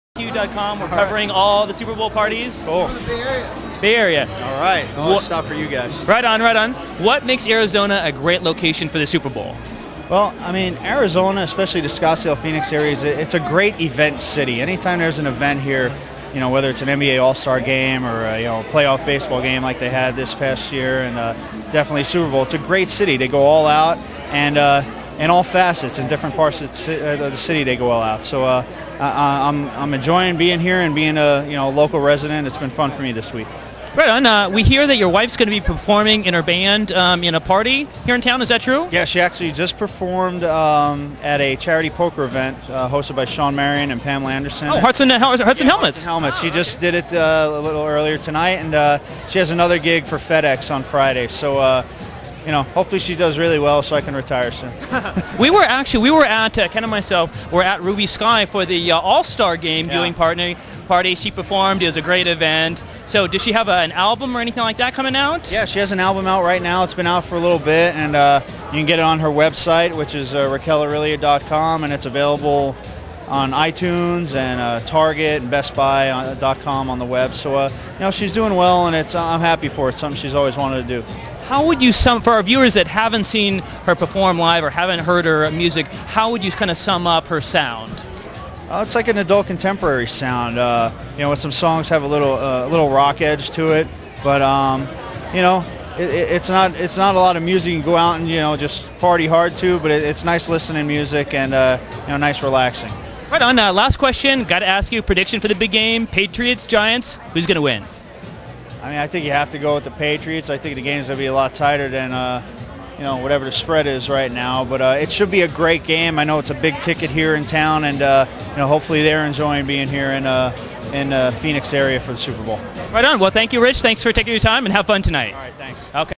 Super Bowl XLII Party Red Carpet Arizona 2008 Rich Aurilia
Rich Aurilia interviewed at the Matt Leinart Super Bowl party in Arizona, January 30 2008